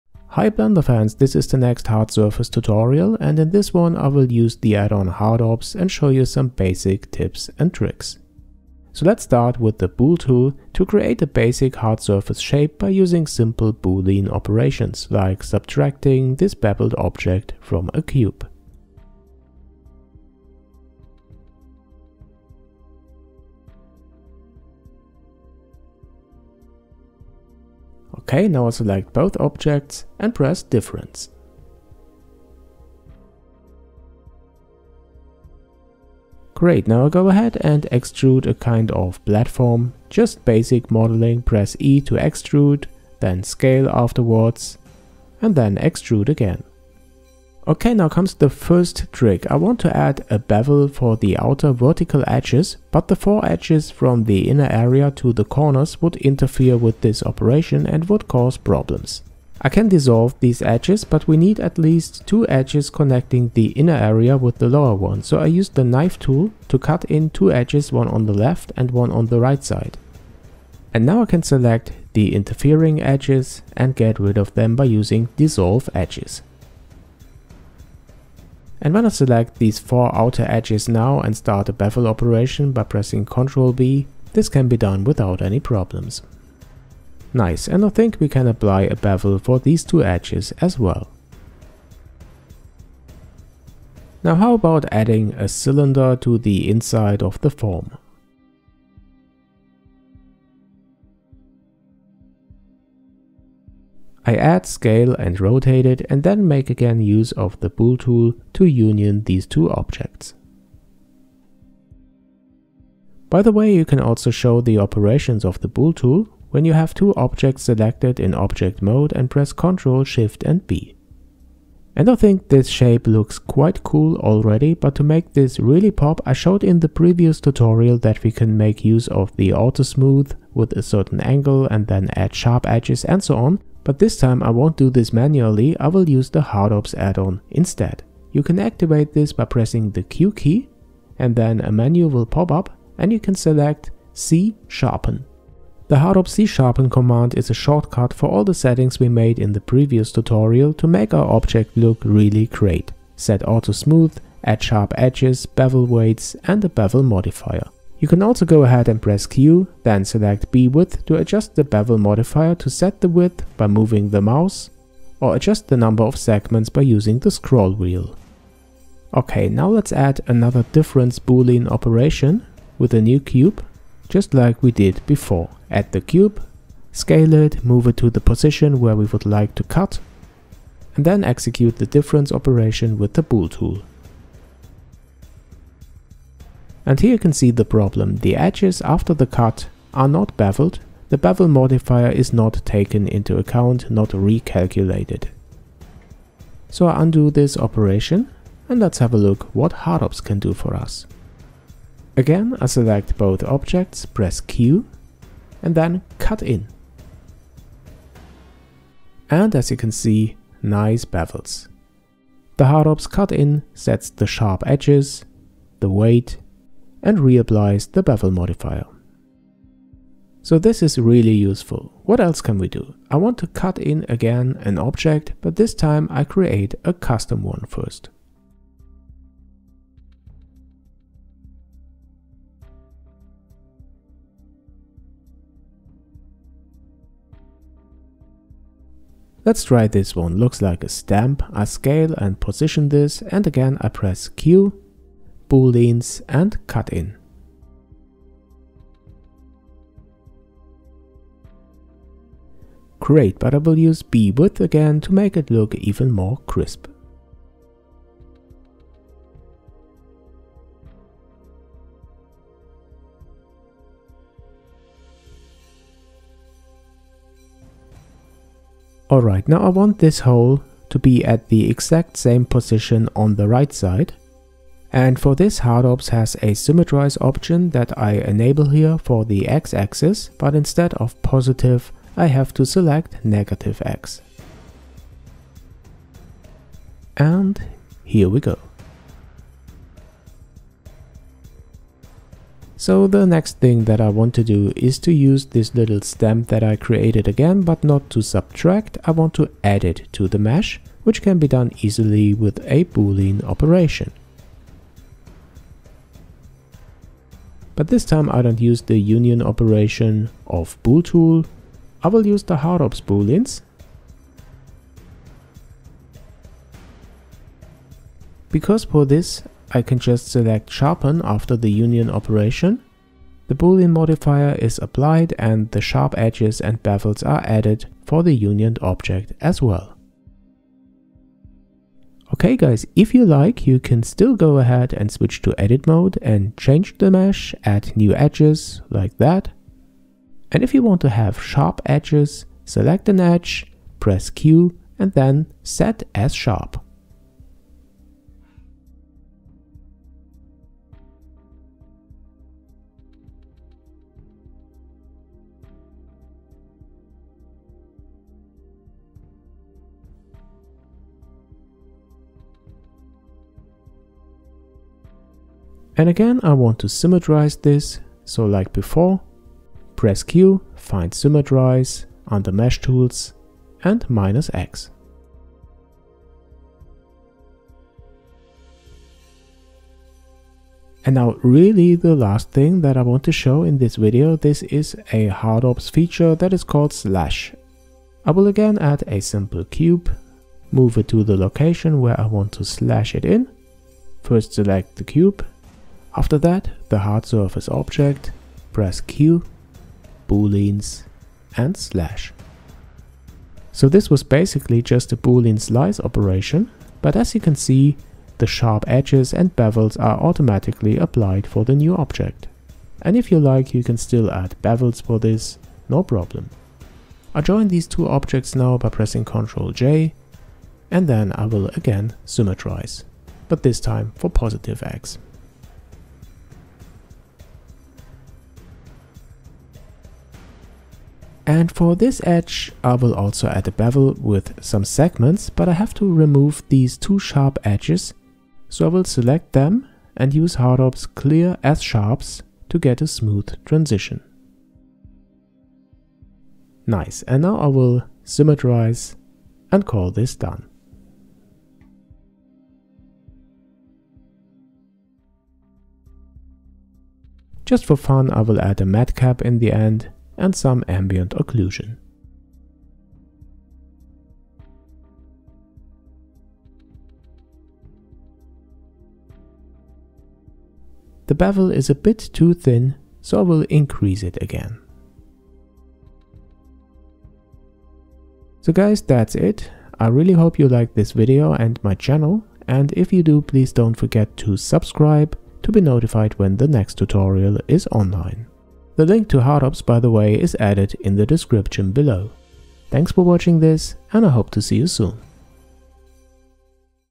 Blender Hard Surface Tips : Hardops
In this Blender hard surface video I show some basic tips & tricks for the Addon hardops version 0.93.